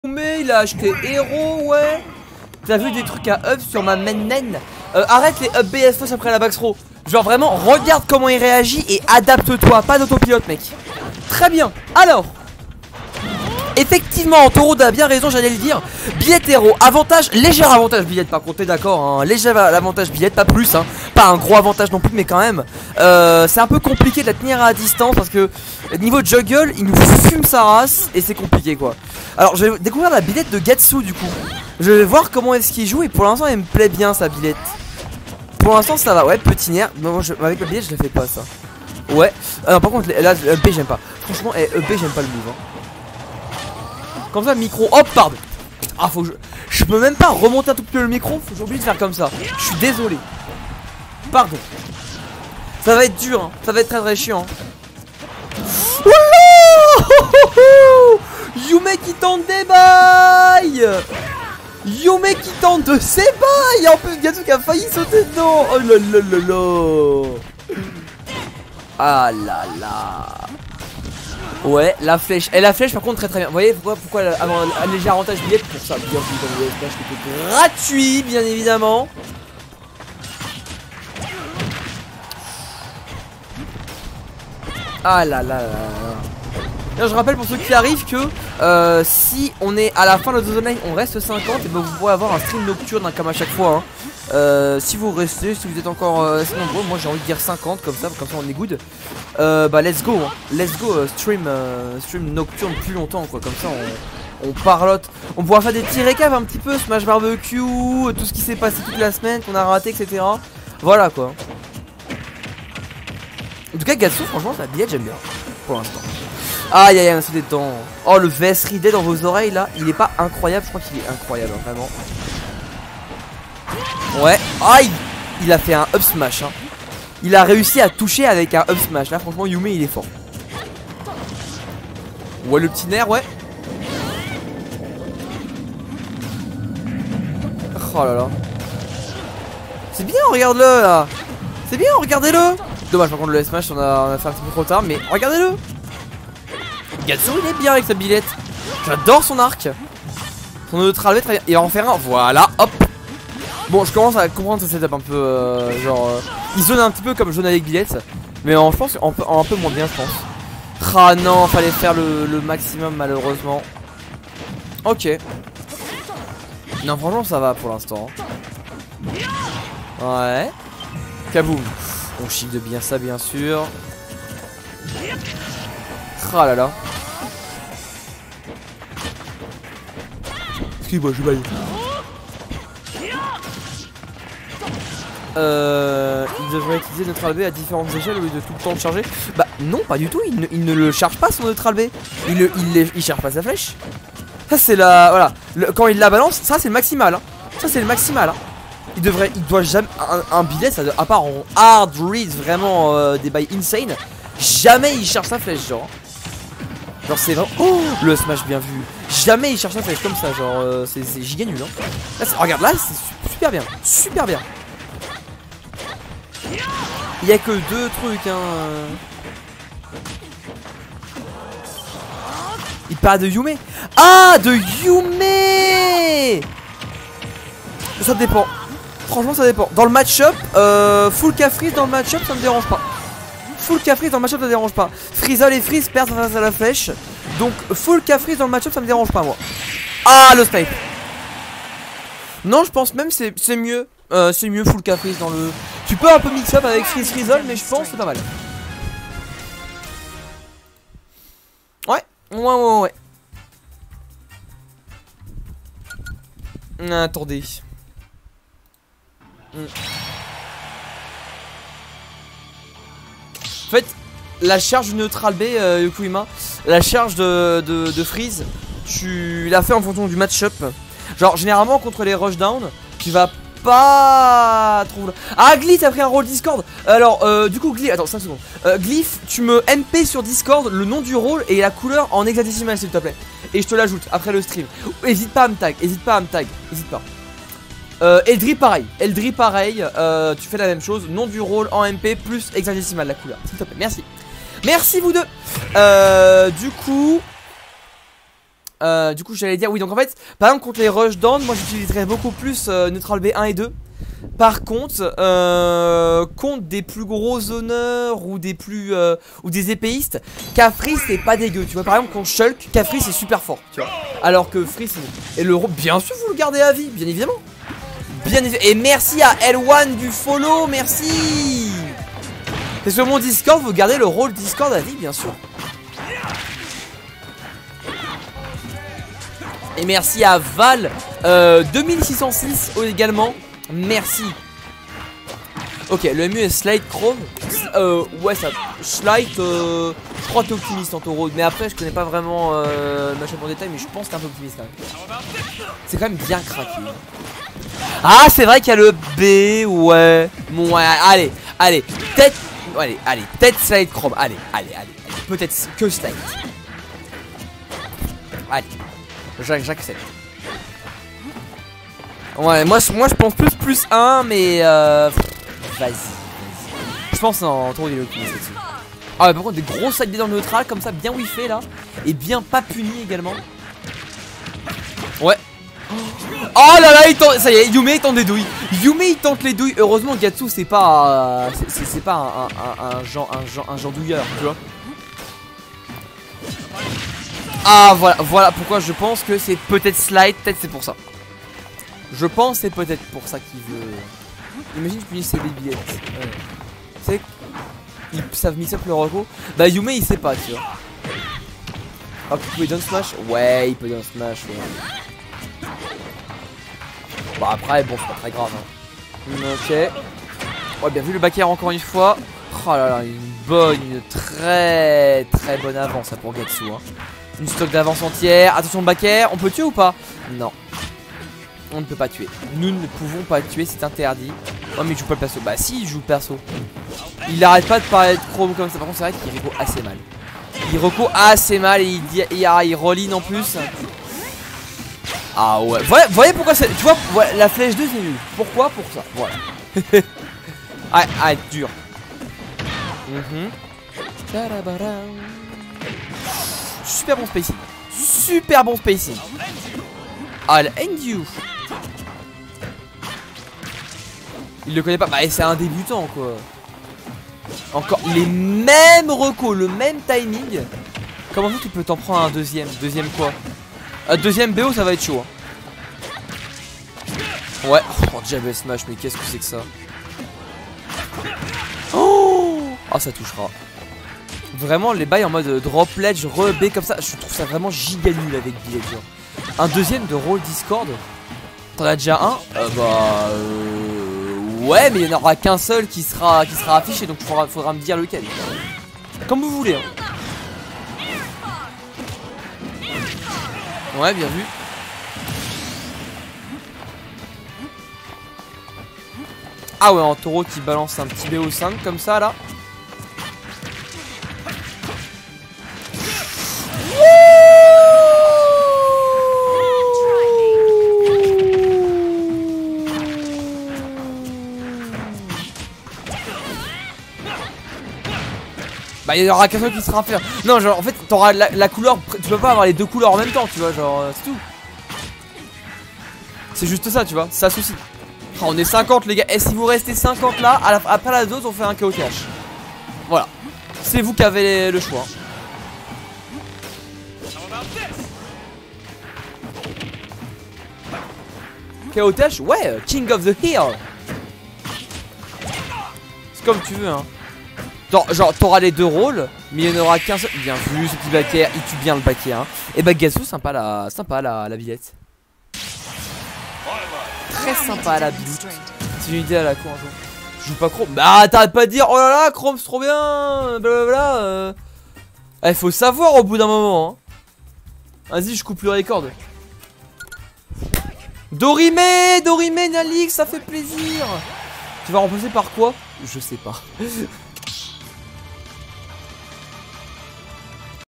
La Dose Online - Gatsu (Byleth) vs Yume (Hero, Chrom)
Eh salut

Rediffusion de La Dose Online du 18/08/2020 avec Gatsu contre Yume !

Si tu veux participer aux stream et retrouver tes sets sur Youtube comme celui-ci, n'hésite pas à prendre connaissance de mon planning en me suivant sur mes réseaux !

All hail Gelul, et à bientôt !